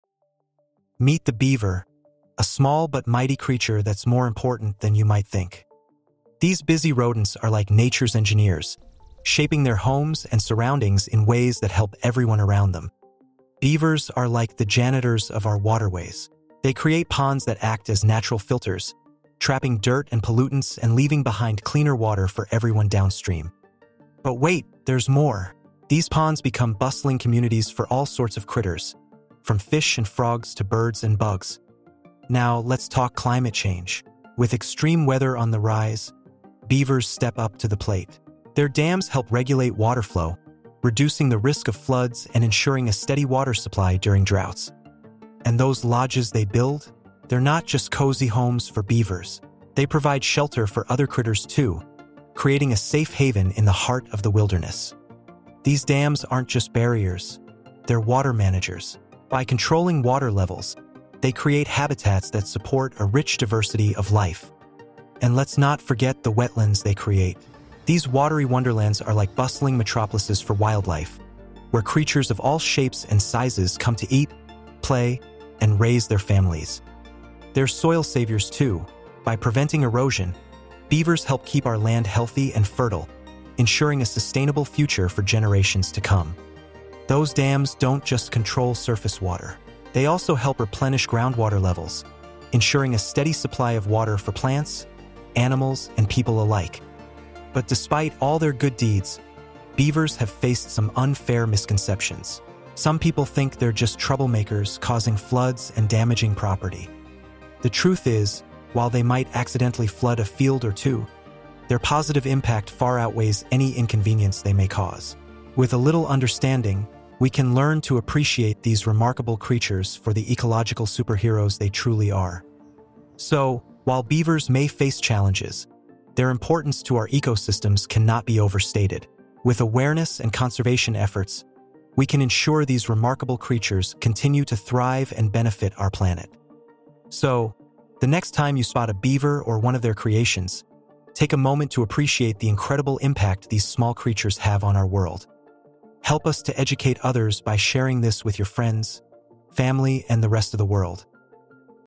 Why Are Beavers Important To The Ecosystem | Beaver | The Planet Voice
Dive into the fascinating world of beavers with our captivating documentary! Discover how these small yet mighty creatures shape entire ecosystems through their incredible engineering skills. From building dams to creating wetland habitats, beavers play a vital role in maintaining the balance of nature. Join us as we explore the hidden wonders of beaver ponds, learn about their impact on water flow regulation, and uncover the surprising ways they help prevent floods and droughts. With stunning footage and engaging narration, this video sheds light on the often-overlooked importance of beavers in our environment. Don't miss out on this journey into the secret world of nature's engineers!